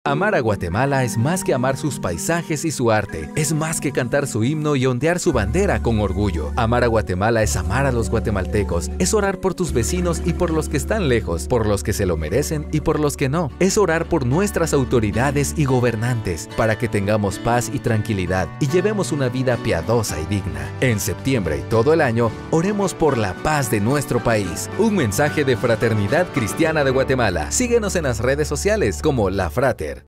Amar a Guatemala es amar a los guatemaltecos - La Fráter
Amar a Guatemala es más que amar sus paisajes y su arte.
Es más que cantar su himno y ondear su bandera con orgullo.

Amar a Guatemala es amar a los guatemaltecos,
es orar por tus vecinos y por los que están lejos,
por los que se lo merecen y por los que no.
Es orar por nuestras autoridades y gobernantes
para que tengamos paz y tranquilidad, y llevemos una vida piadosa y digna.
En septiembre y todo el año, oremos por la paz de nuestro país.